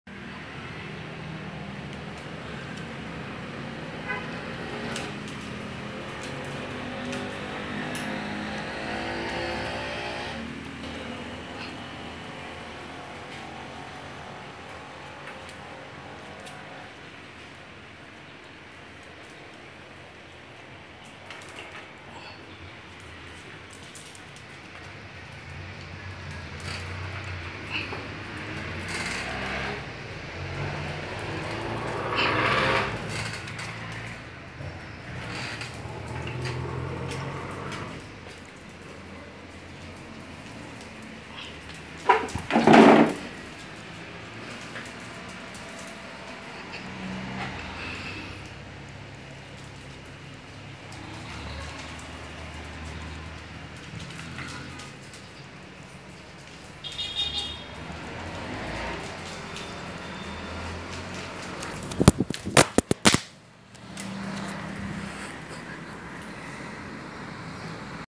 Amber the dog, its stuff toy and the effects of dropped mobile phone😁😆😆
Amber the dog, its stuff toy and the VFX(joke) of dropped mobile phone.😁😆😆